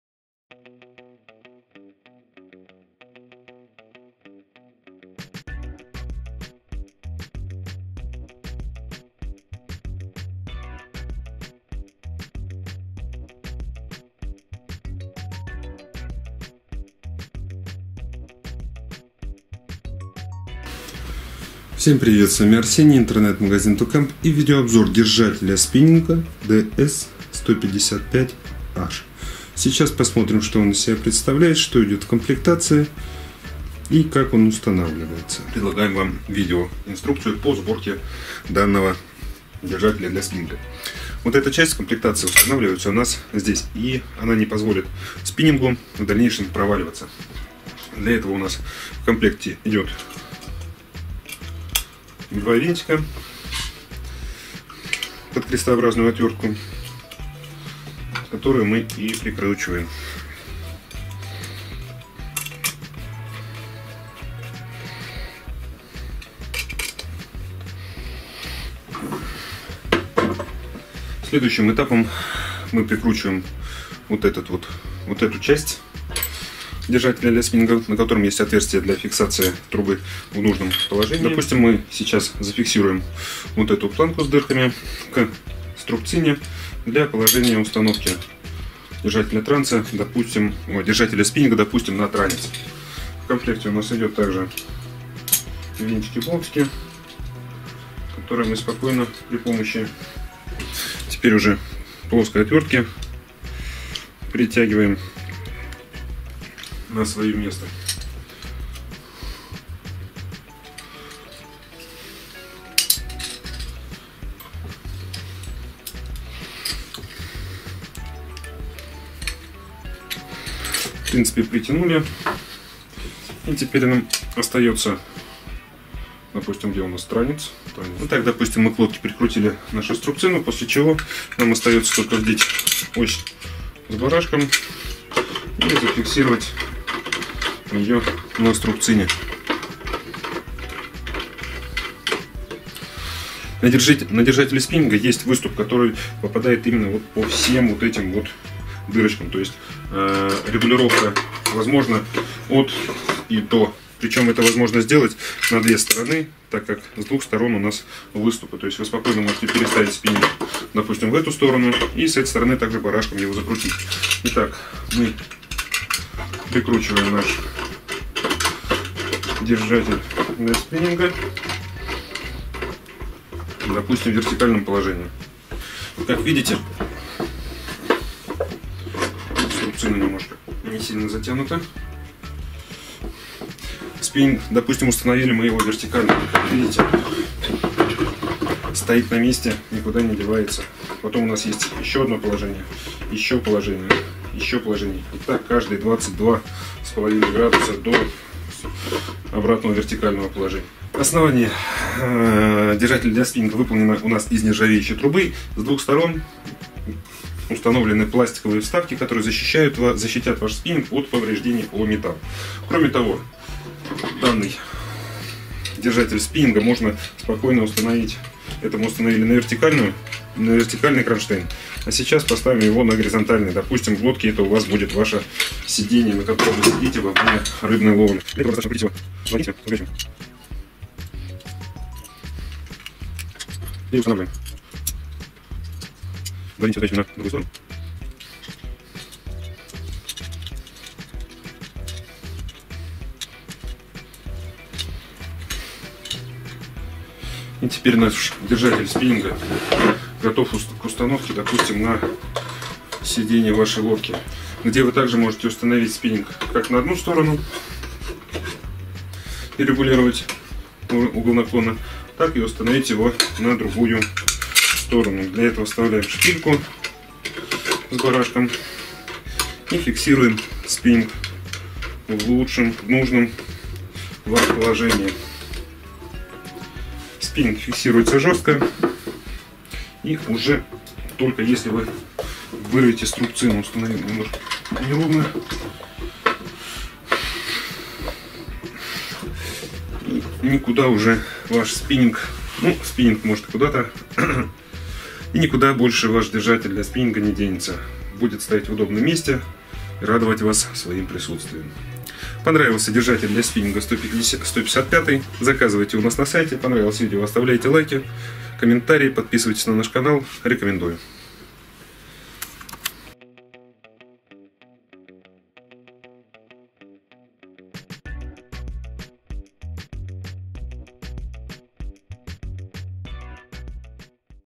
Держатель для спиннинга в лодку ПВХ DS 155H
Держатель для спиннинга DS-155, это универсальный держатель для крепления любых спиннингов и удилищ на борту вашего судна, будь то катер или лодка ПВХ. Данная модель позволяет стационарно зафиксировать держатель для спиннинга в удобном для вас месте на горизонтальной или вертикальной поверхности транца или сиденья лодки. Если вы используете струбцину, то по толщине она захватывает до 30 мм. Регулируется держатель по наклону практически на 360 градусов. Простота конструкции, отечественное производство позволило сделать цену максимально привлекательной, что обеспечивает хороший спрос данного товара.